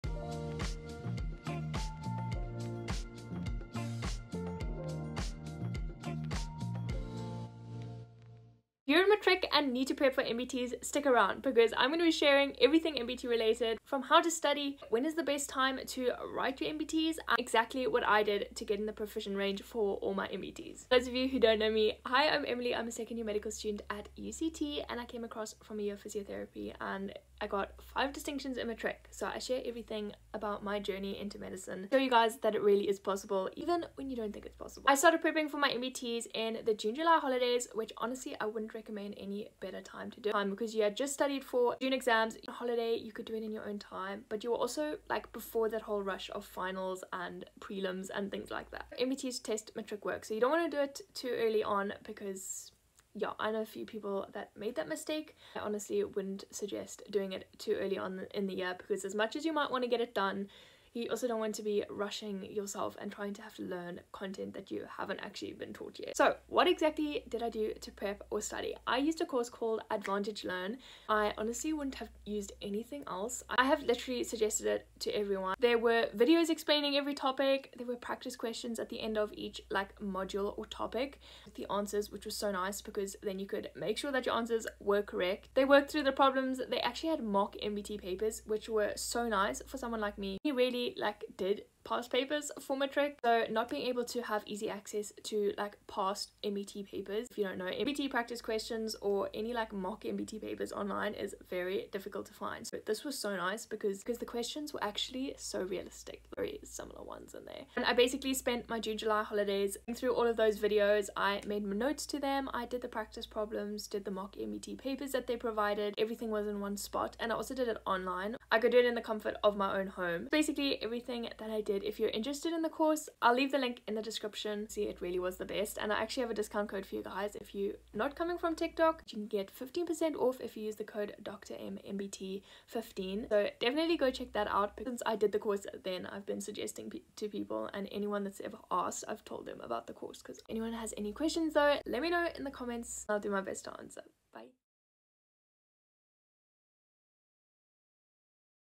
Everything you need to know to prep for NBT’s! (How to study, when to write & best resources!)
Sharing everything I did to prep for NBT’s as a South African medical student! Best time to write, how I studied, what resources I used and more!

(Clickable link is in comment section!)

xx Emily